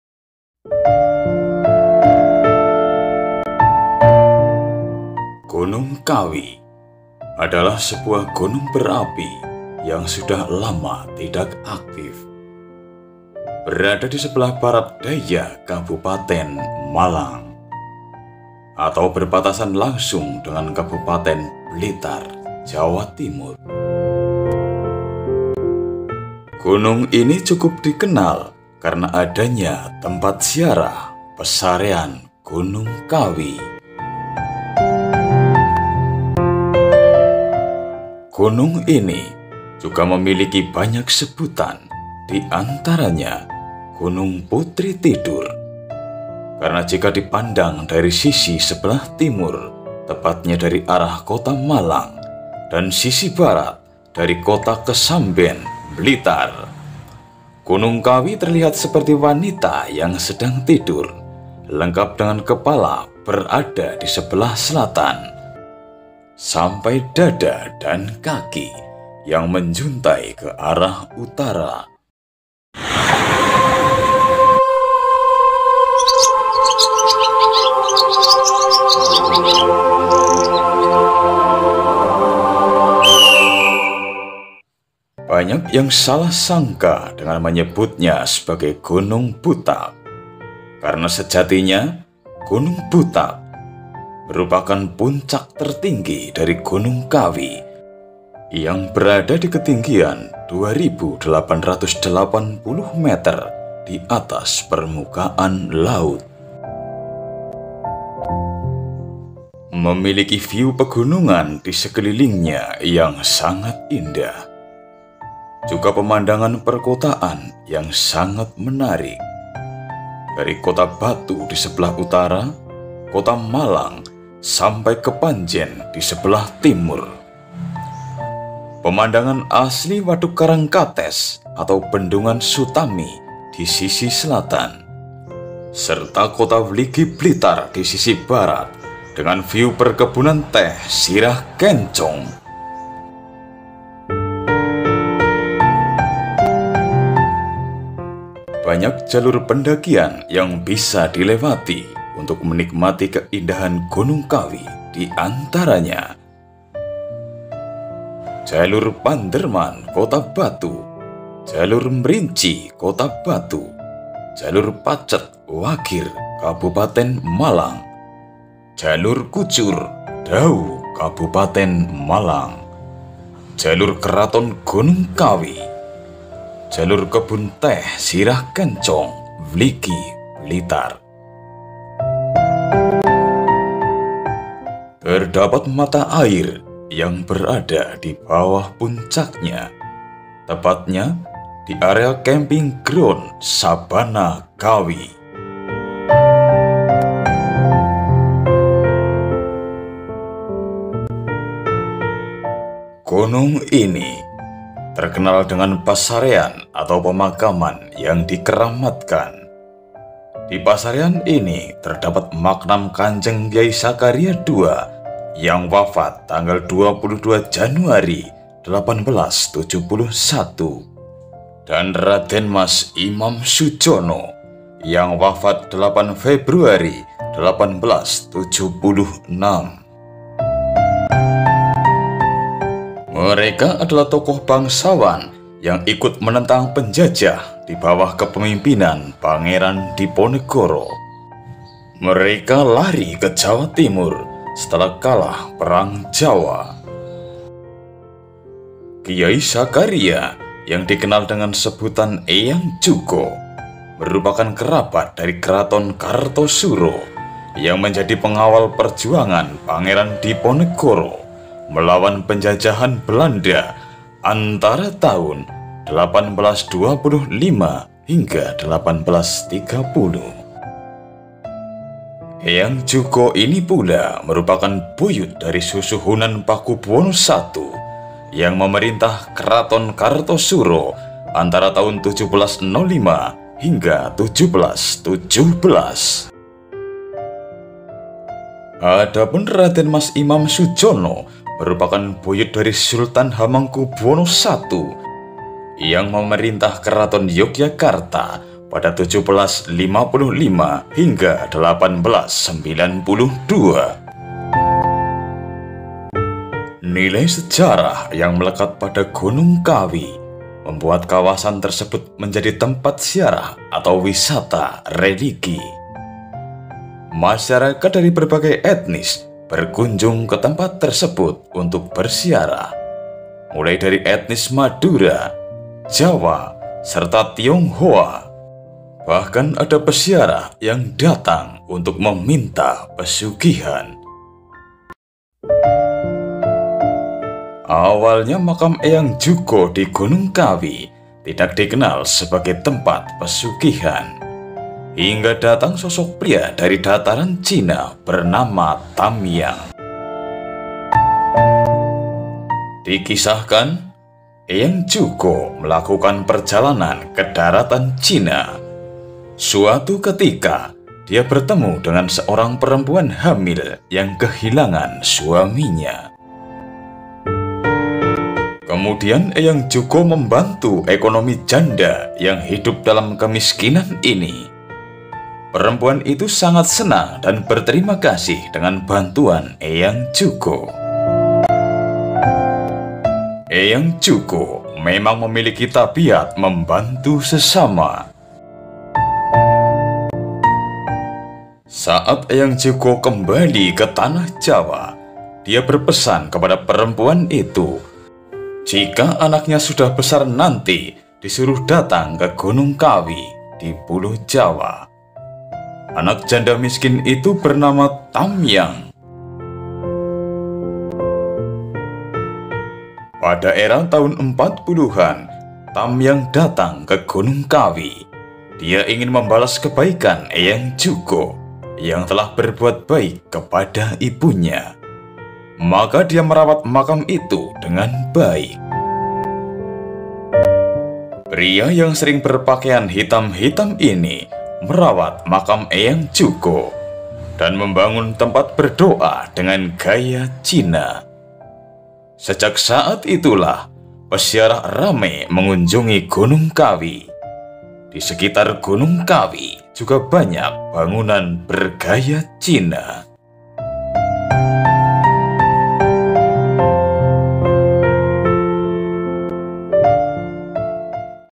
ngalap berkah mitos atau fakta,ritual pesugihan gunung kawi
wisata religi di gunung kawi,dari sejarah perjuangan hingga pengharapan untuk hidup yang lebih baik#gunungkawi#malang#blitar#jawatimur#wisatareligi#pesugihan#mbahjugo#alam#kebunteh#segaramadu@SegaraMadu108